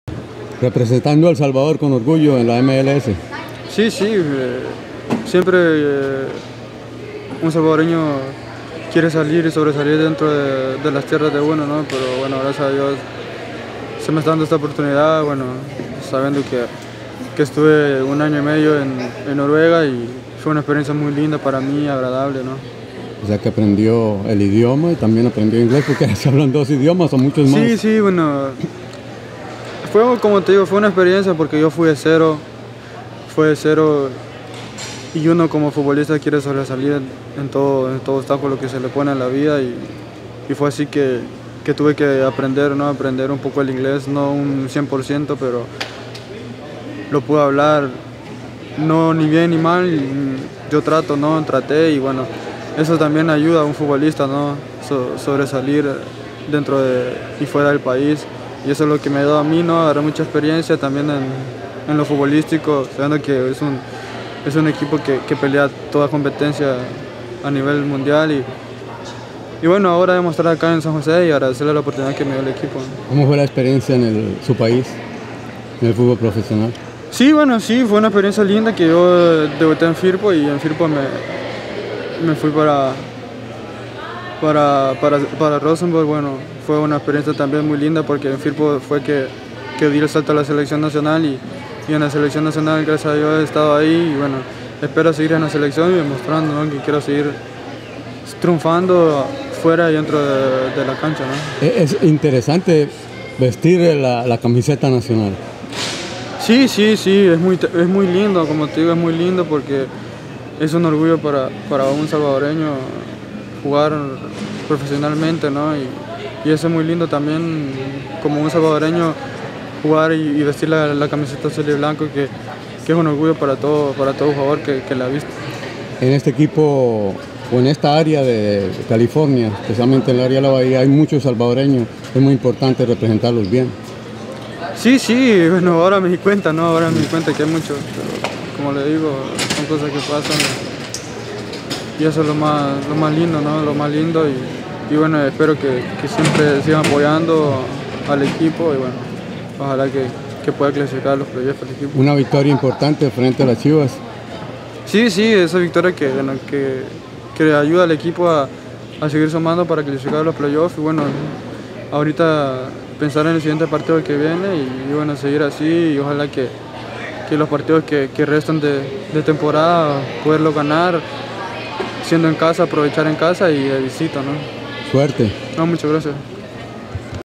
Jaime Alas, mediocampista salvadoreño de los Terremotos, habla sobre la MLS.
.Jaime Alas, mediocampista salvadoreño de los Terremotos de San José, habla sobre su participación en la MLS, de su experiencia en el balompié noruego y el gran deseo de regresar a la selección de El Salvador para las eliminatorias del mundial 2014 que se realizará en Brasil.

Alas ha representado a El Salvador en 33 partidos internacionales y aparece con 6 anotaciones. Su primera actuación internacional fue en un partido amistoso contra Panamá, que se jugó en 2010. El primer gol internacional lo anotó en la Copa Centroamericana frente a la selección de Nicaragua.

Jaime fue miembro del fútbol profesional de El Salvador con el onceno Luis Ángel Firpo.